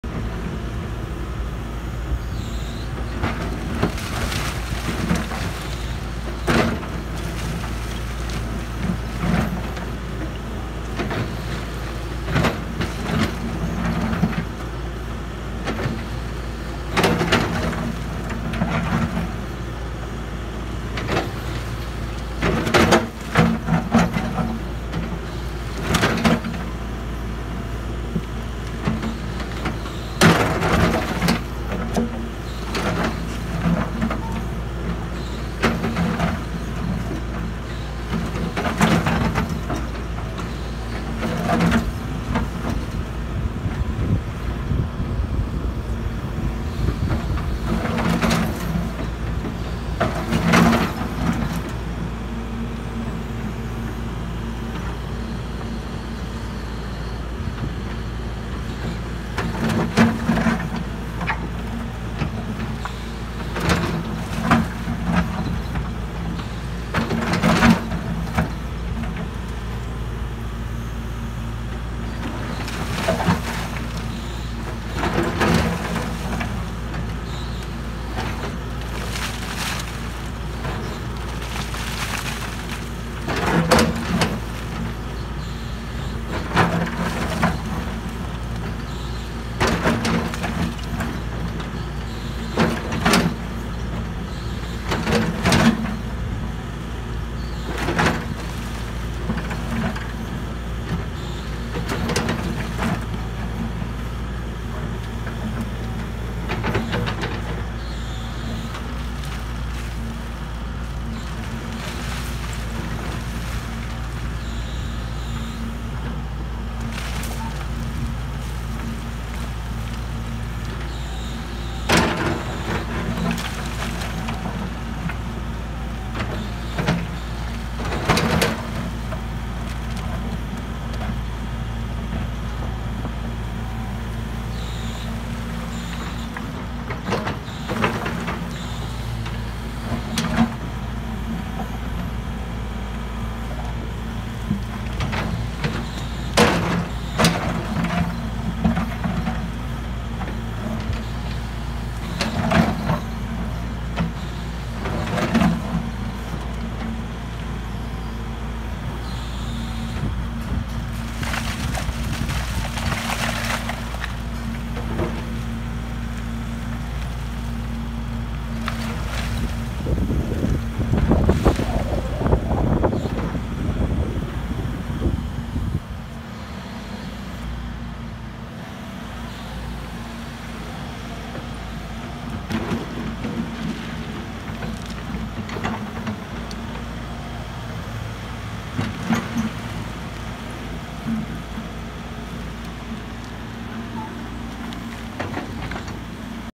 SANY SY135C ESKAVATÖR DÜNYA MARKASI OLMA YOLUNDA✅HAFRİYAT ÇIKARIYORUZ.🤙🏻eskavator
BU VİDEOMUZDA SANY SY135C  BELKİ DE HİÇ GÖRMEDİĞİNİZ BİR ESKAVATÖR SERİ İŞMAKİNASI İLE HAFRİYAT ÇIKARIYORUZ👌🏻✅